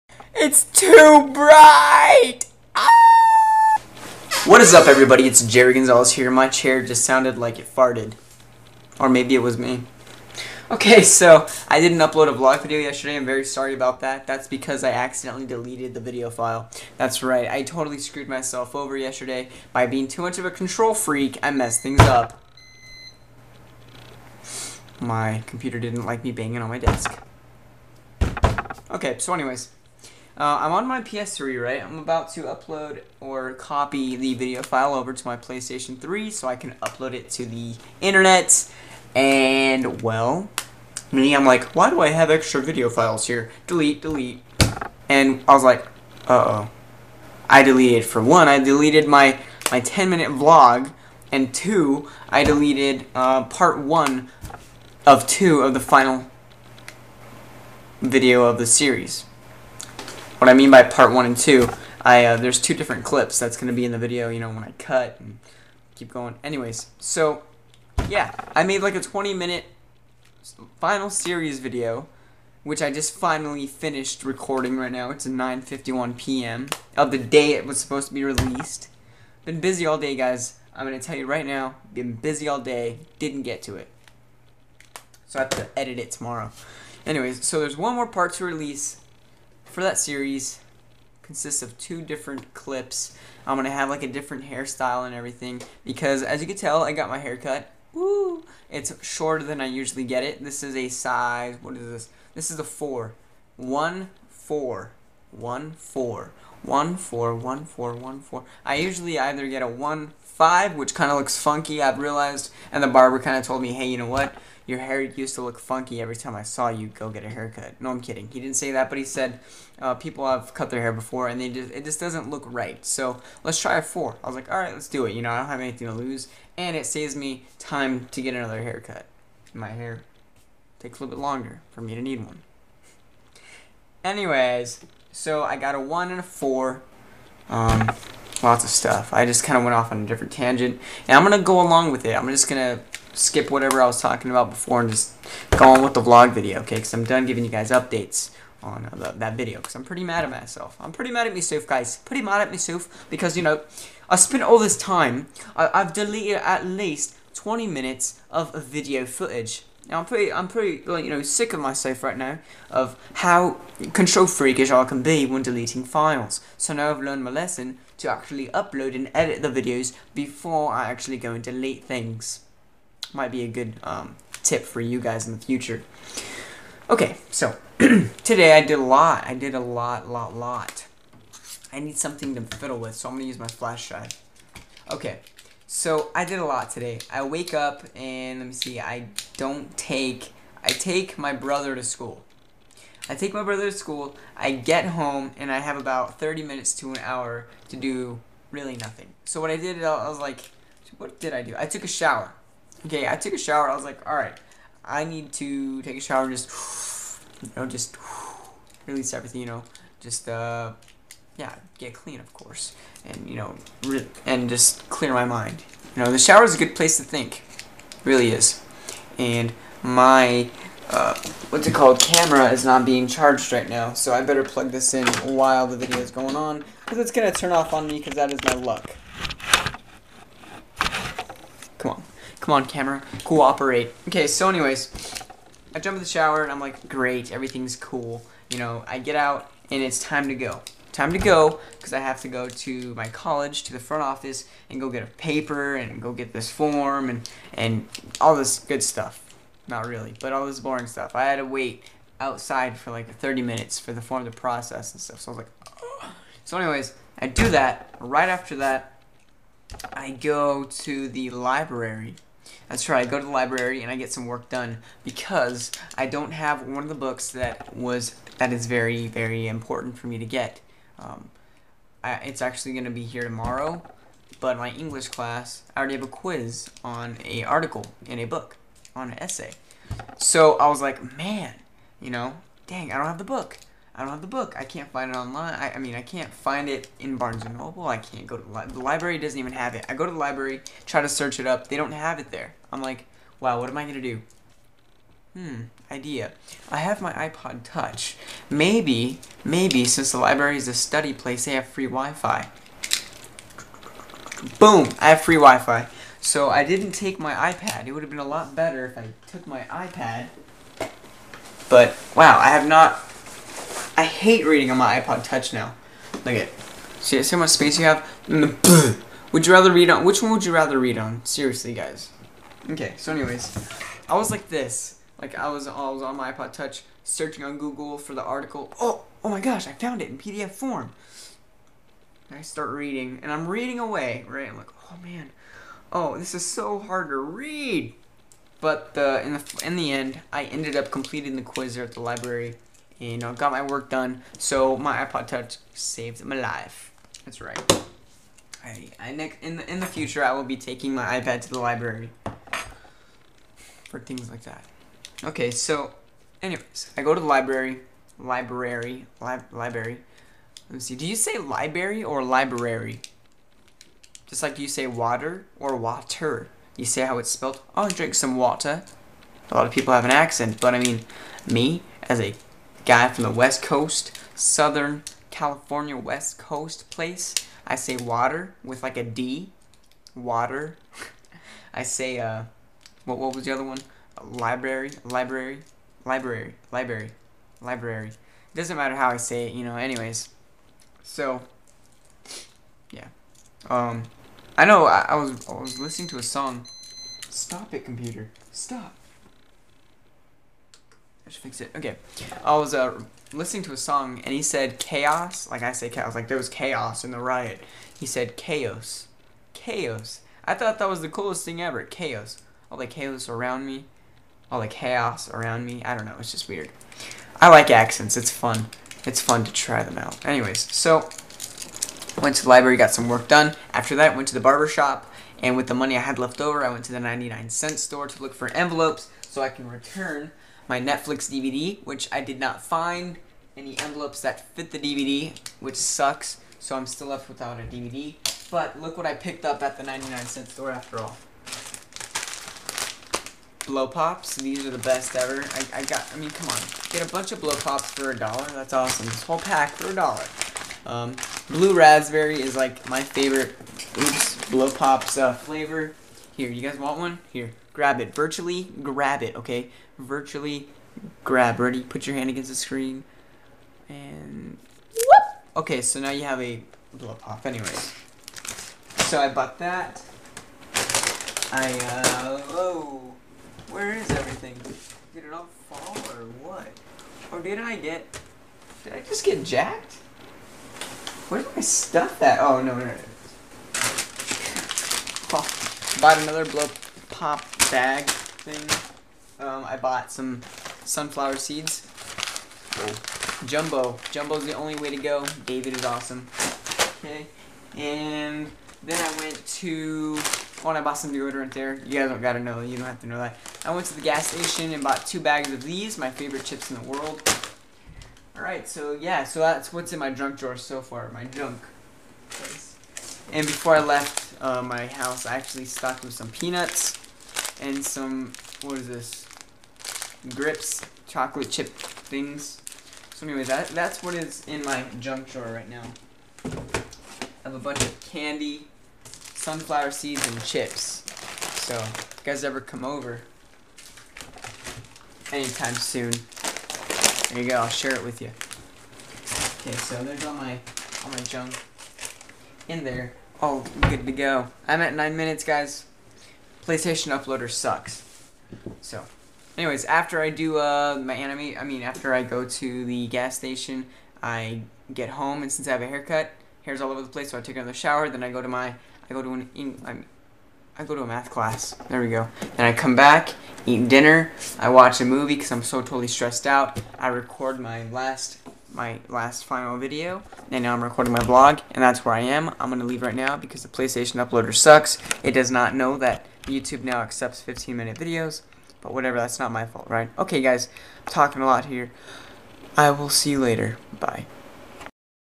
What Words Have You Heard People Pronounce Differently?
Alrighty...today was really busy. I go over what happened to my vlog video yesterday and the final series video updates. also my day. 8)